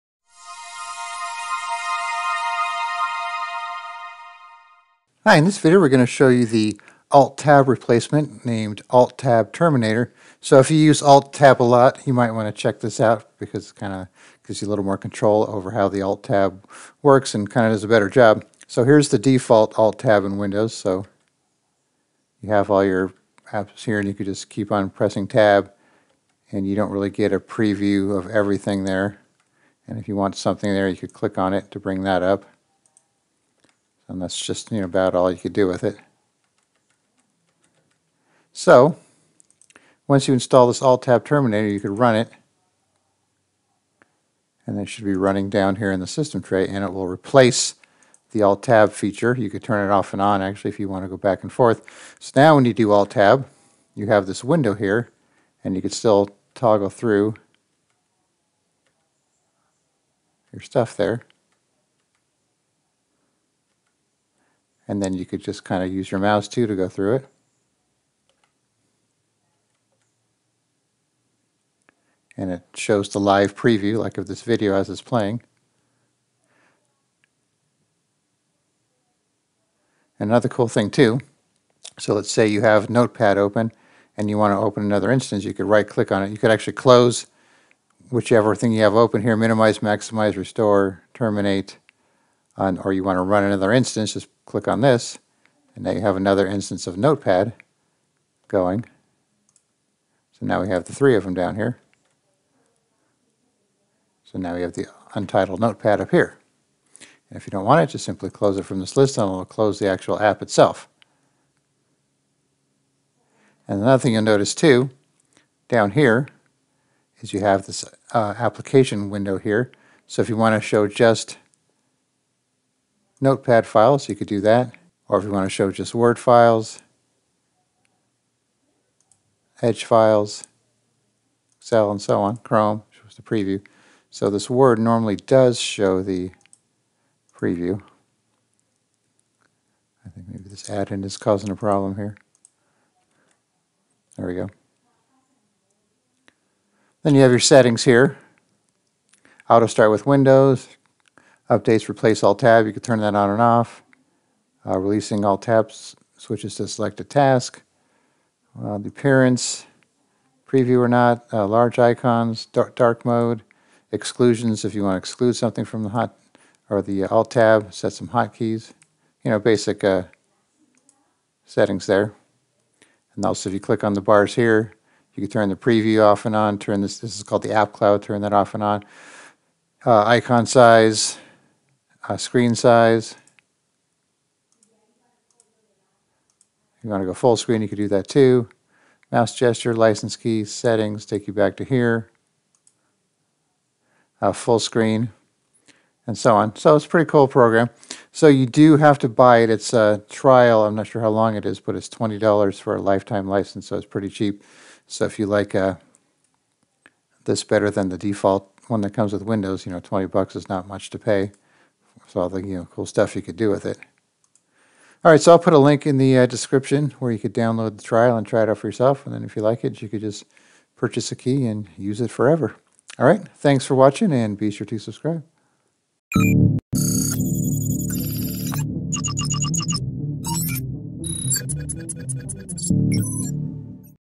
Alt-Tab Terminator Windows Alt-Tab Replacement App
If you are a Windows power user who relies on the Alt-Tab feature to cycle through all of your open apps and programs so you can easily switch between them, you might want to try out the Alt-Tab Terminator app that replaces the default Windows Alt-Tab function. It offers real time previews of your open apps and you can also use the App Cloud to filter which programs are showing in the window. Plus you can minimize, maximize and close programs right from the app itself and even open new instances of your software.

Here is a link to download the Alt-Tab Terminator software.